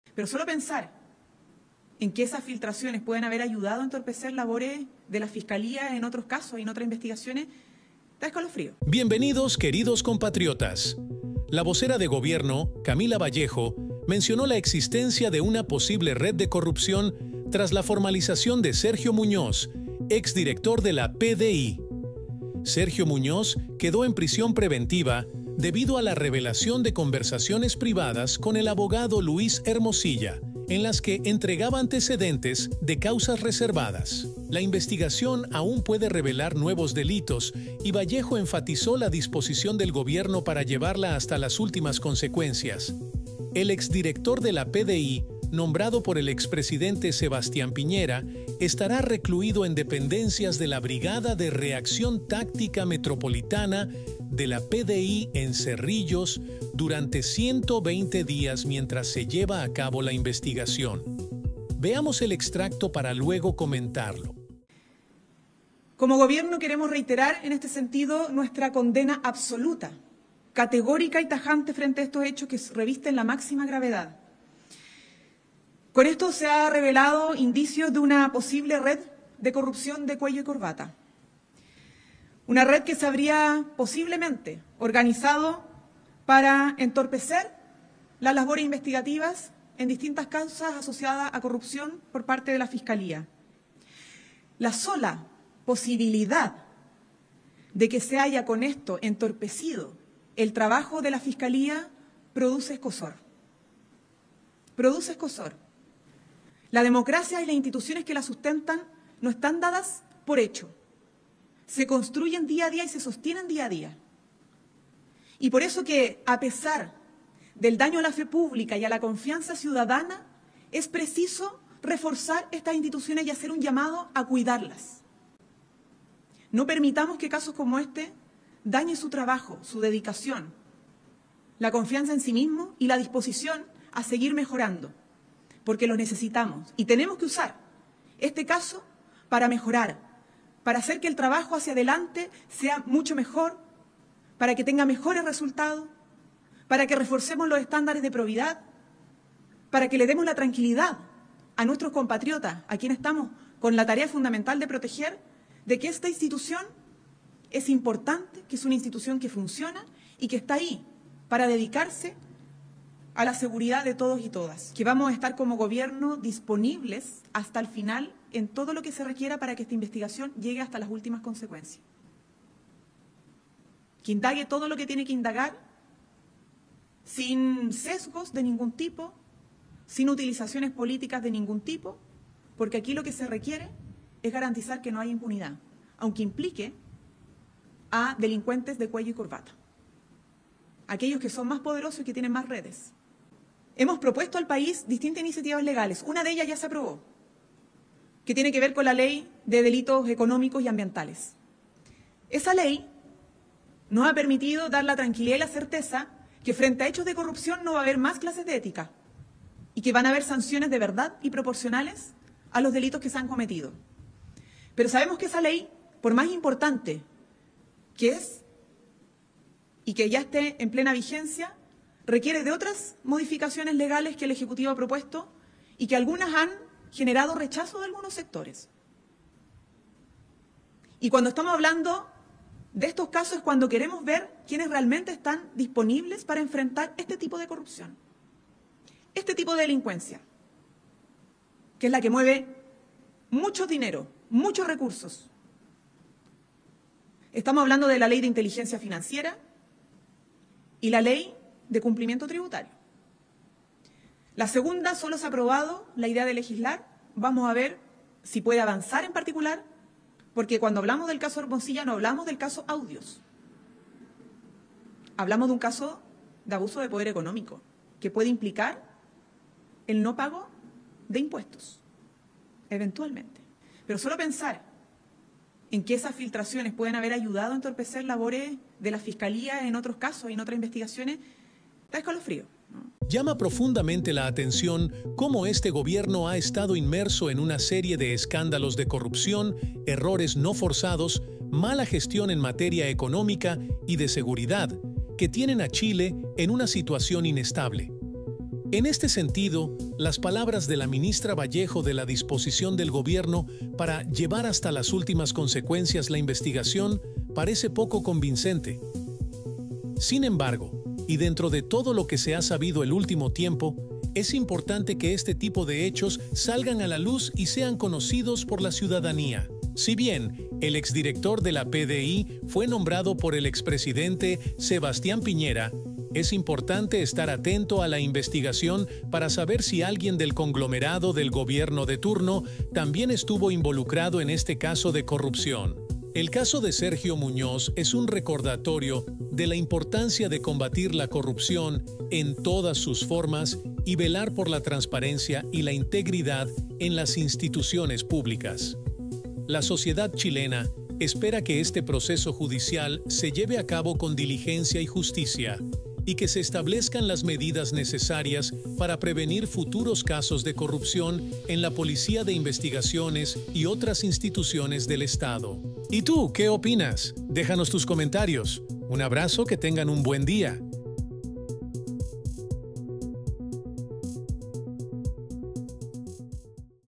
MINISTRA VALLEJO SE REFIERE A PRISIÓN PREVENTIVA DE EX DIRECTOR PDI
La vocera de Gobierno, Camila Vallejo, mencionó la existencia de una posible red de corrupción tras la formalización de Sergio Muñoz, exdirector de la PDI.

Sergio Muñoz quedó en prisión preventiva debido a la revelación de conversaciones privadas con el abogado Luis Hermosilla, en las que entregaba antecedentes de causas reservadas.

La investigación aún puede revelar nuevos delitos y Vallejo enfatizó la disposición del Gobierno para llevarla hasta las últimas consecuencias.

El exdirector de la PDI, nombrado por el expresidente Sebastián Piñera, estará recluido en dependencias de la Brigada de Reacción Táctica Metropolitana de la PDI en Cerrillos durante 120 días mientras se lleva a cabo la investigación.